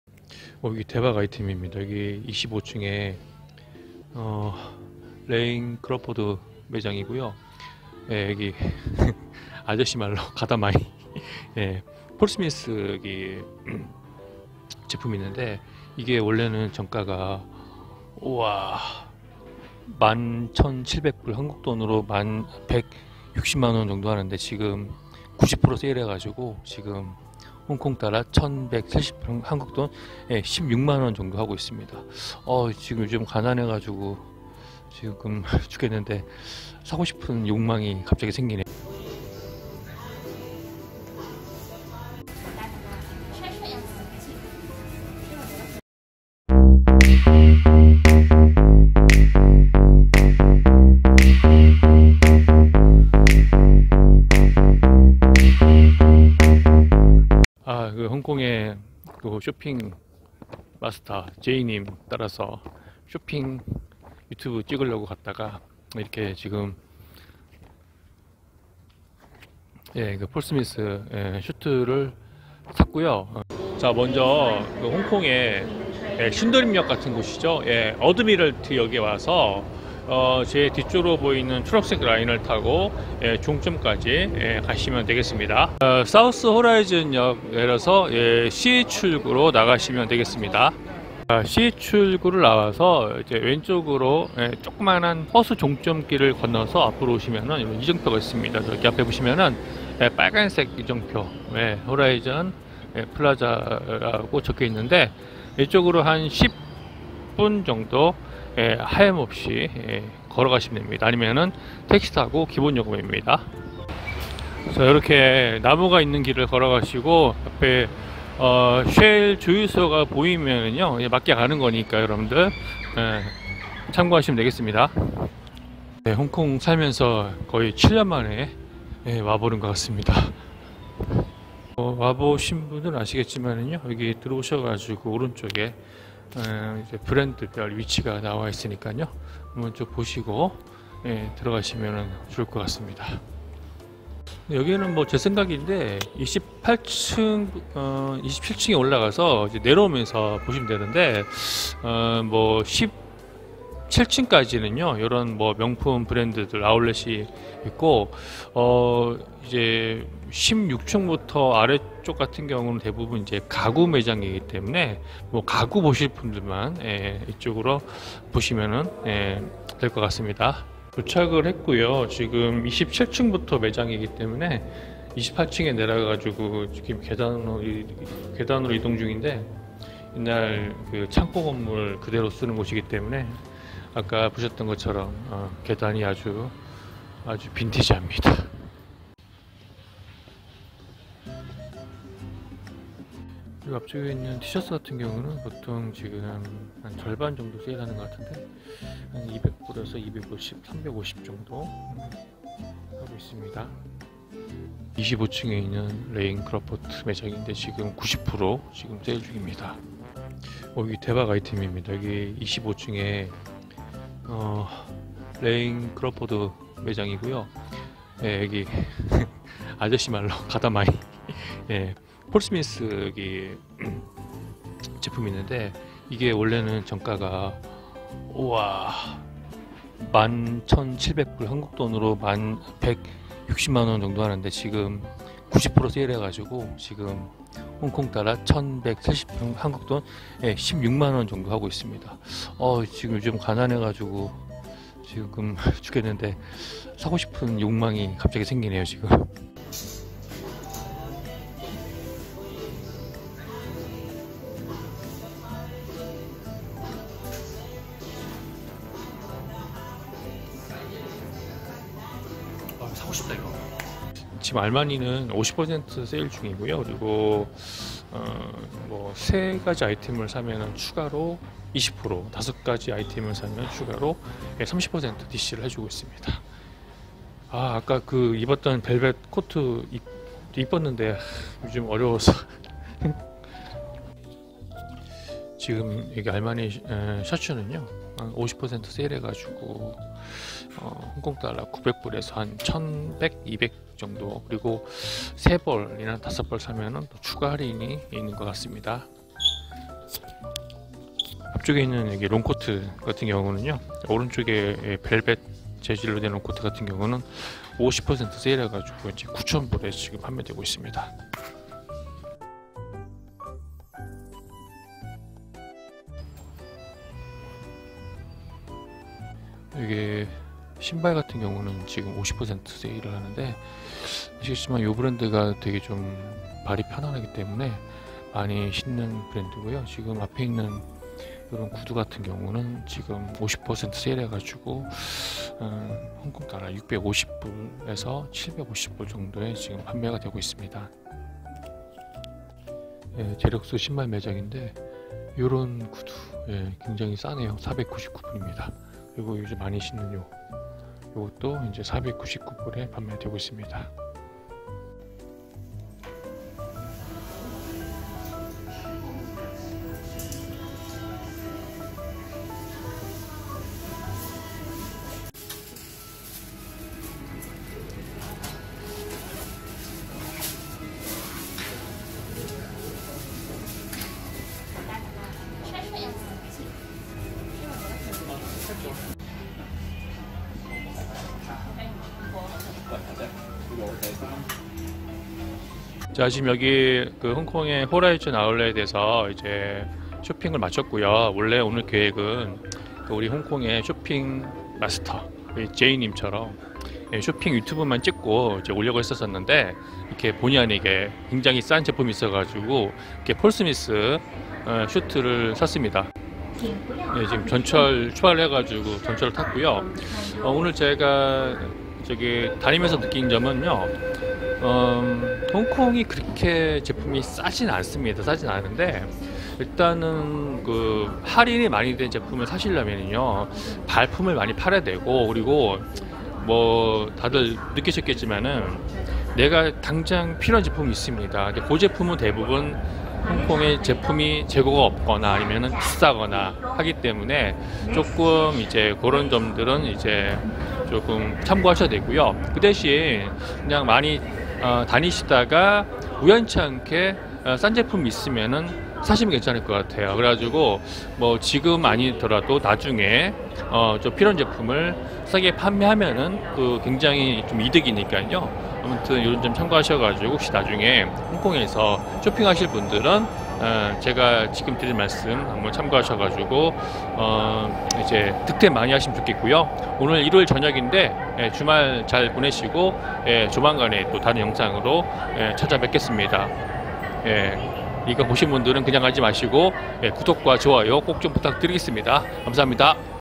홍콩 호라이즌 플라자 폴스미스 블레이져 90% 할인, 17만원에 구입 | Hong Kong Horizon Plaza | Paul Smith |홍콩쇼핑 | 짠한 홍콩쇼핑
홍콩 호라이즌 플라자 폴스미스 블레이져 90% 할인, 17만원에 구입, Hong Kong Horizon Plaza, Paul Smith, 홍콩쇼핑

안녕하세요!!!